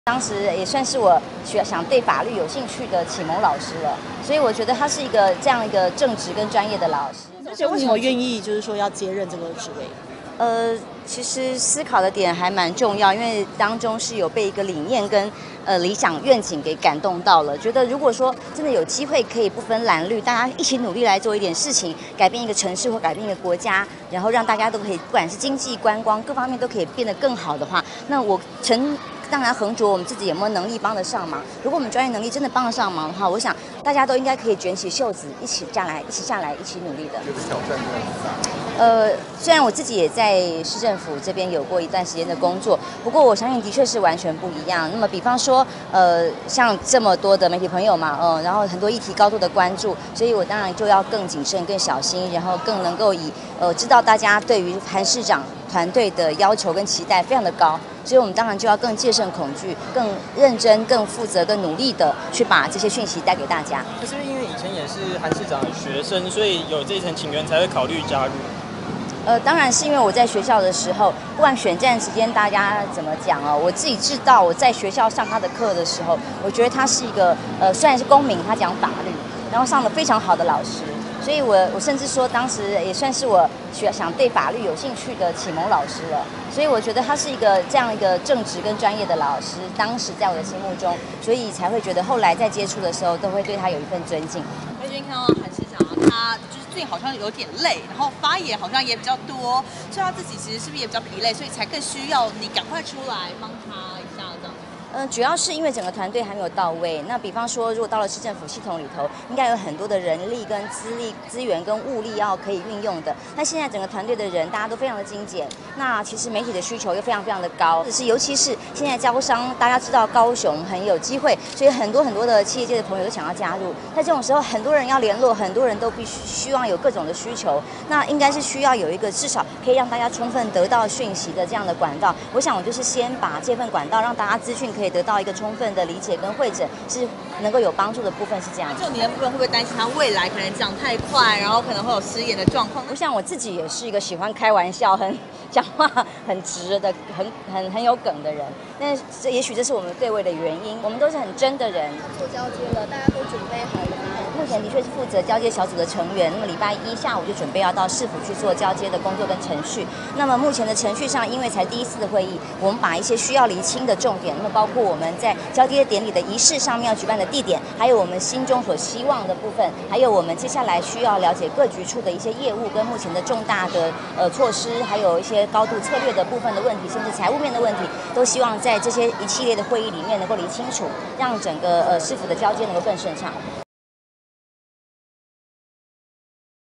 即將正式接任高雄新聞局長 王淺秋：韓國瑜是我法律啟蒙老師
將接任高雄新聞局長　王淺秋：韓國瑜是我法律啟蒙老師
☛即新聞粉絲專頁
☛合作洽談信箱 （或私訊即新聞粉絲專頁）